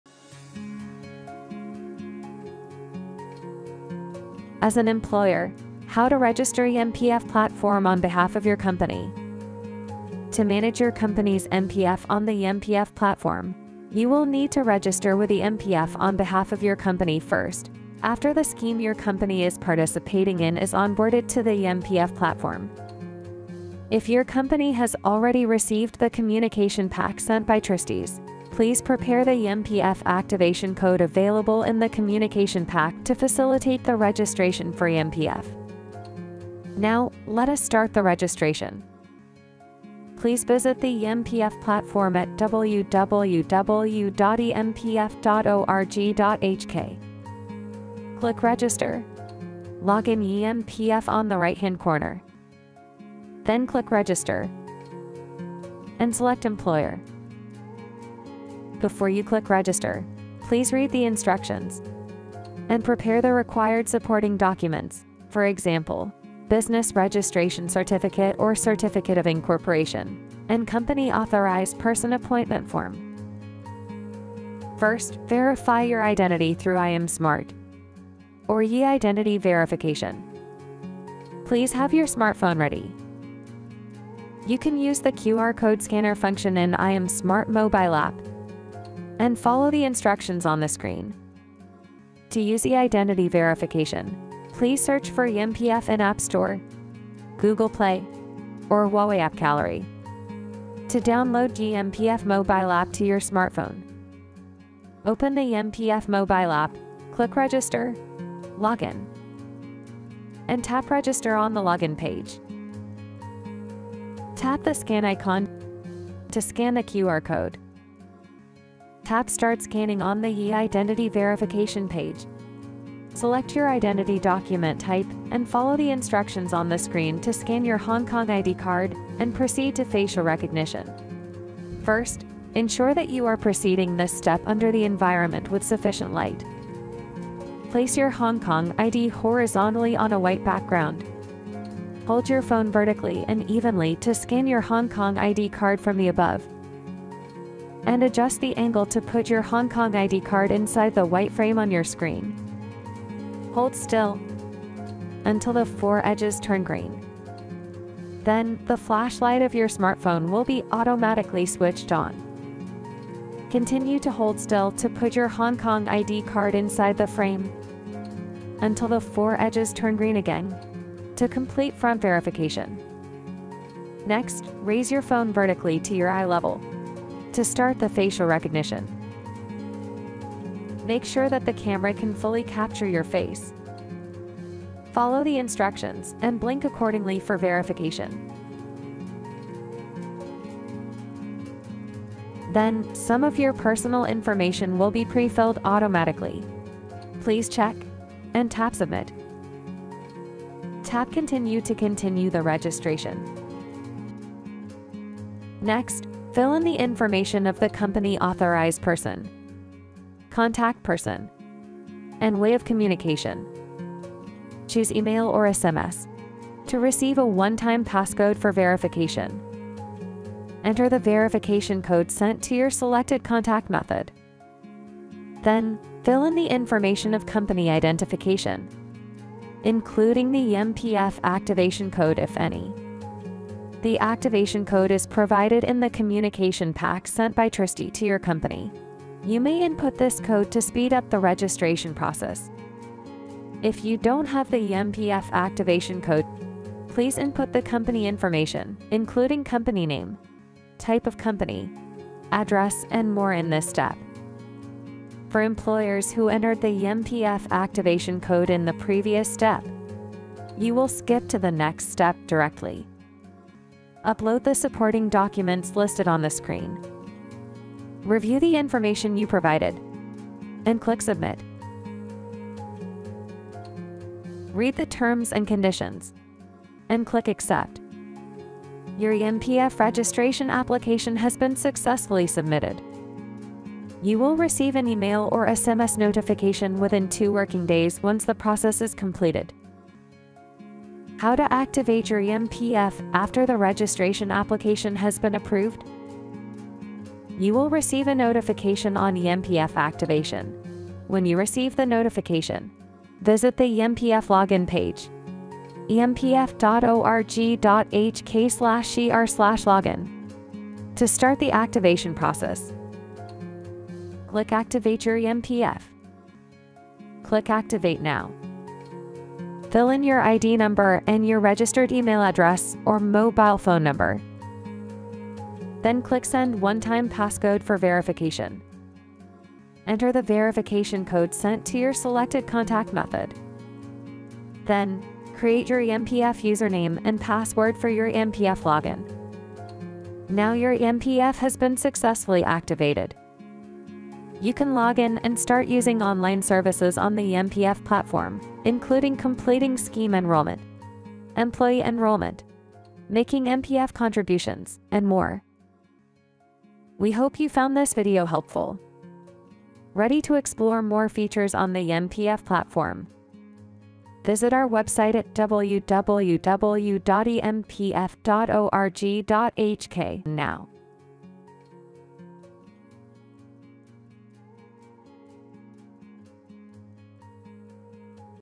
[Employer] How to Register eMPF? - Web Portal
This video illustrates the steps for an employer to register for eMPF on the eMPF Web Portal.